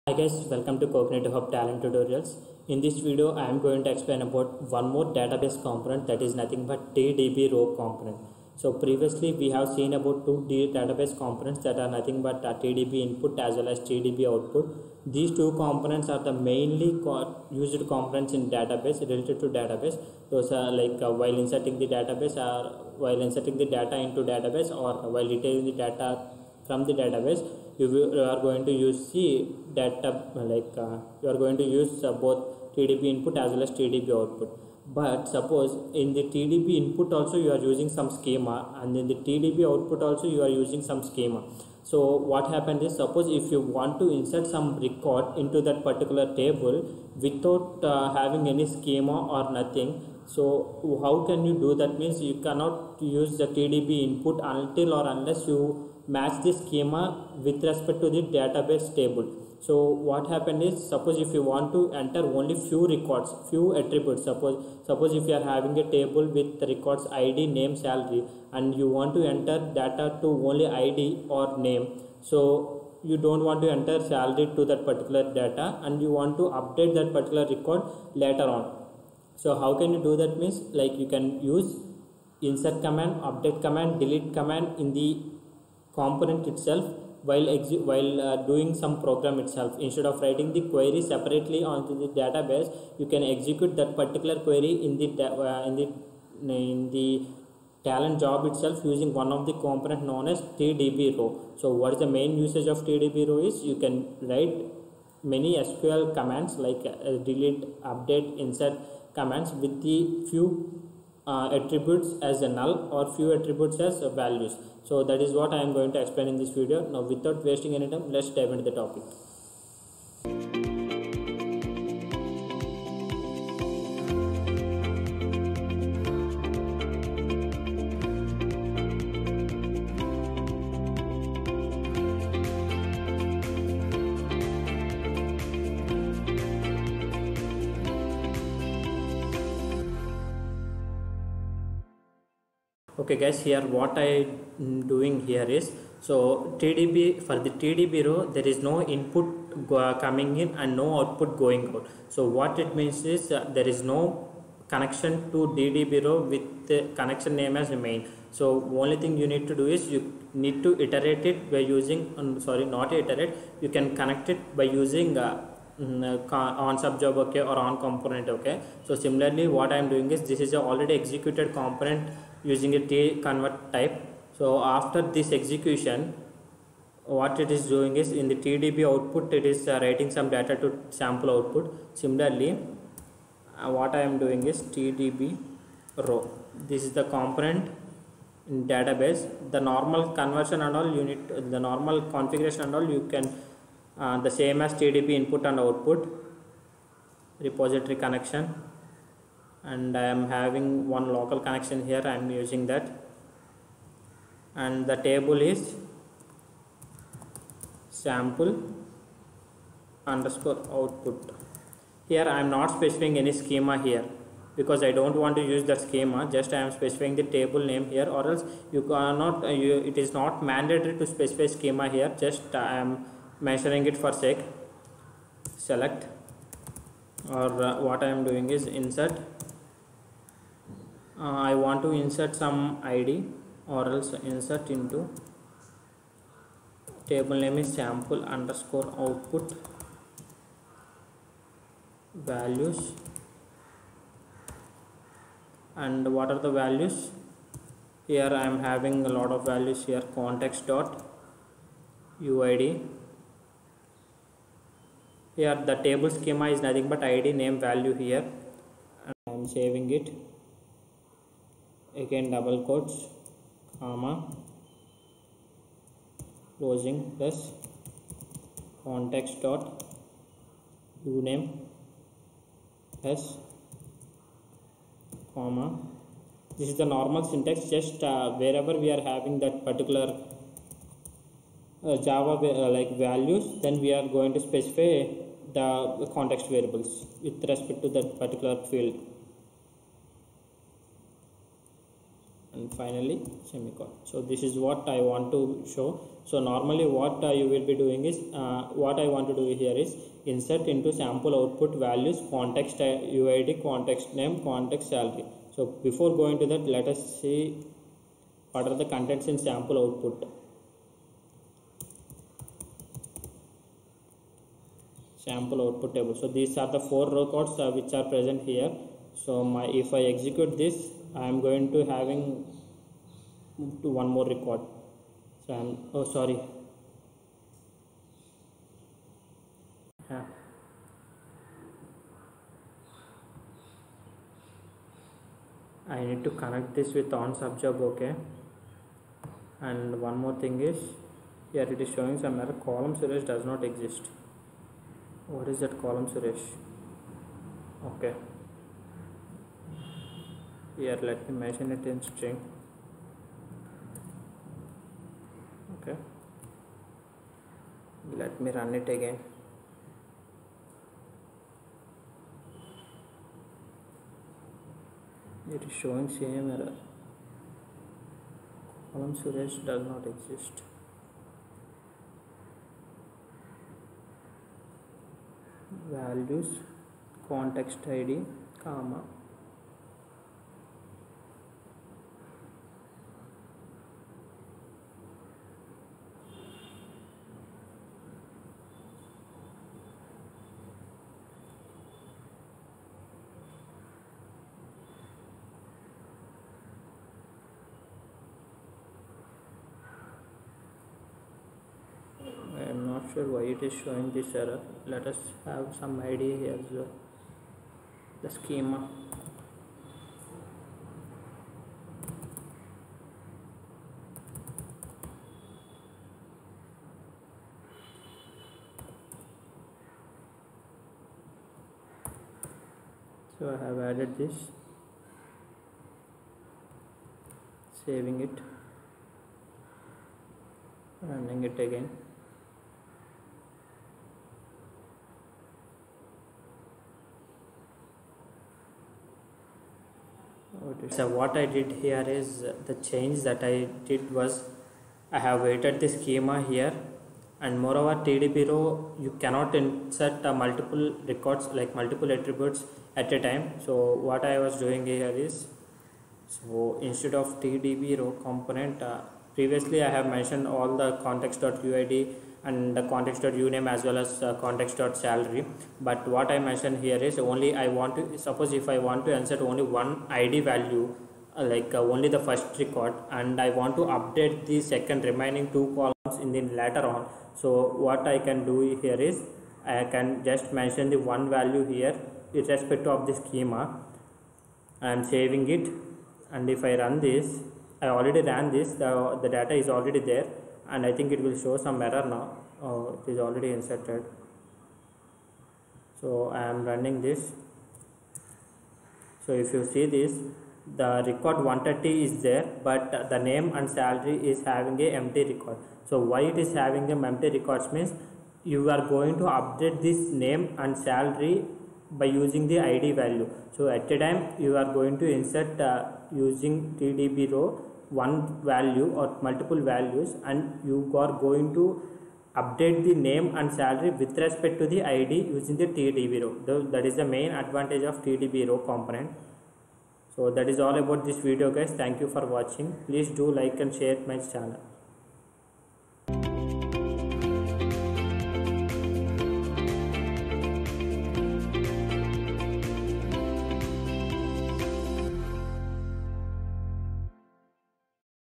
57.How to use tDBRow Component in Talend l tDBRow l Talend Data Integration l Databases
Talend Tutorials

This video covers about how to use tDBRow component in Talend.

If you have any doubts then please drop a message in my linkedin page(Link is mentioned below)

OR

If you have any doubts,Please leave a comment.I will reply back in 48hrs